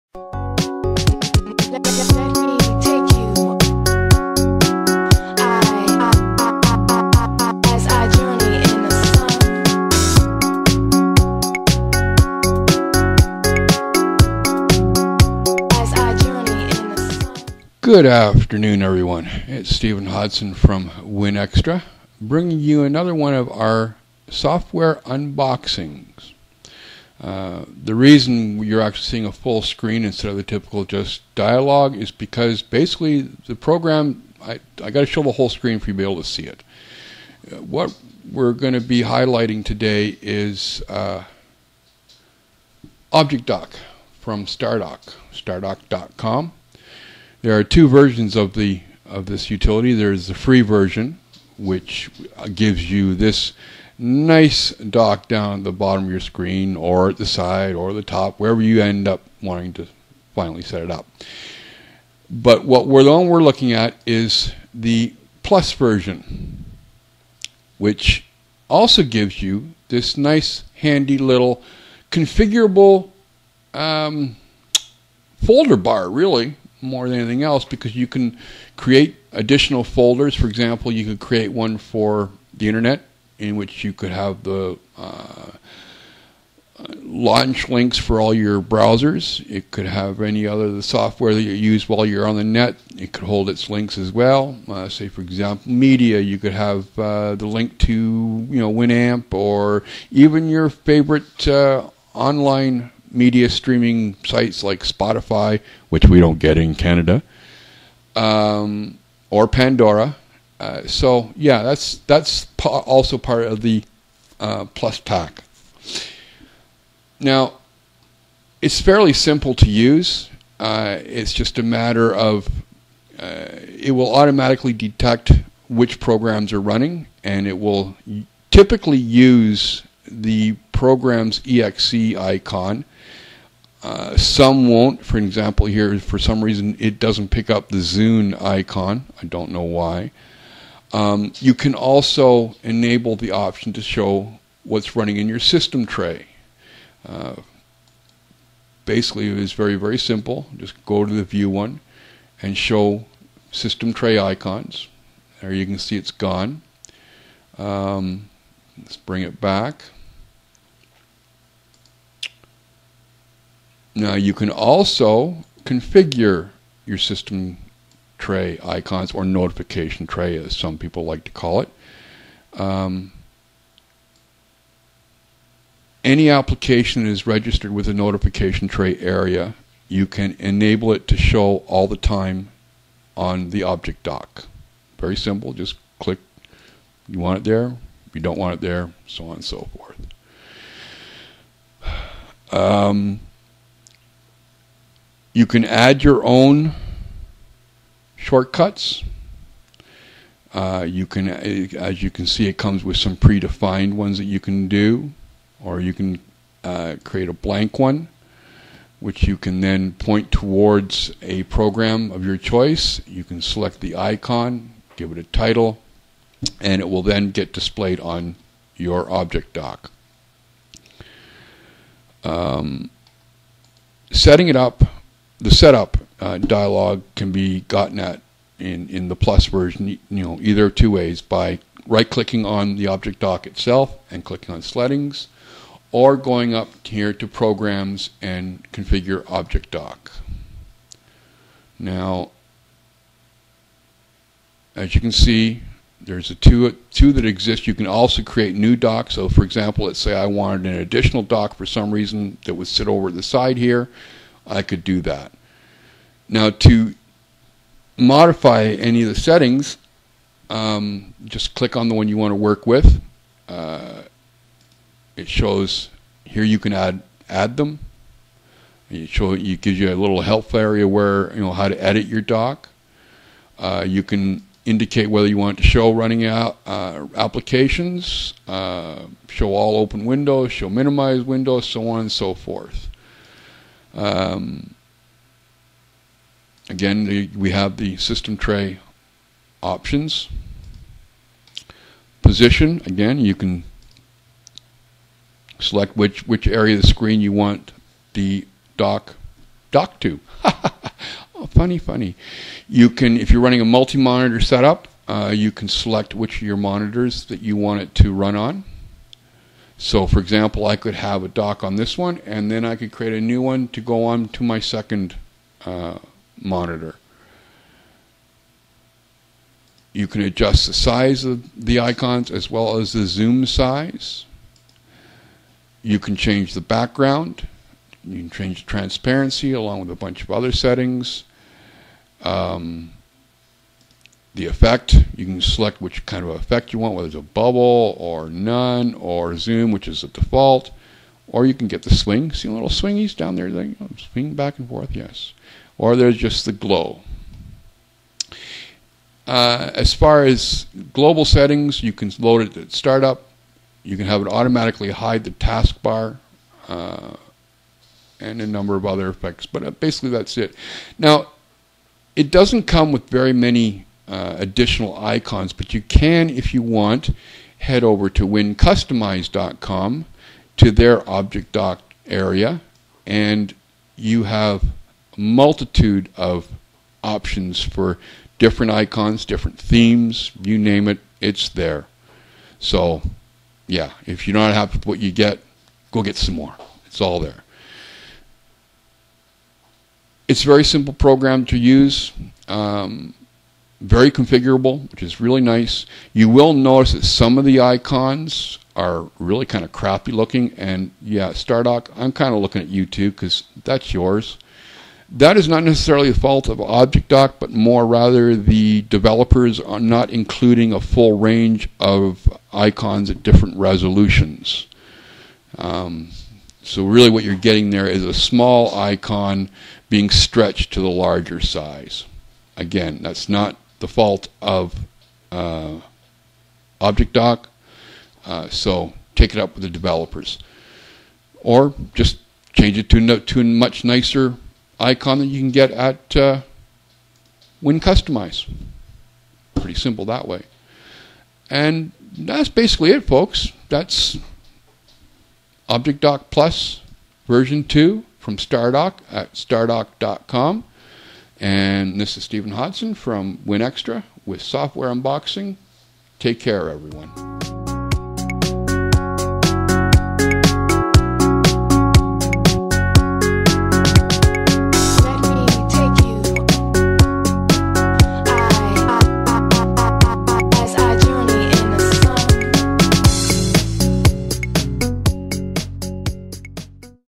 Software Unboxing - Stardock's ObjectDock Plus v2
An overview of Stardock's ObjectDock Plus program launcher.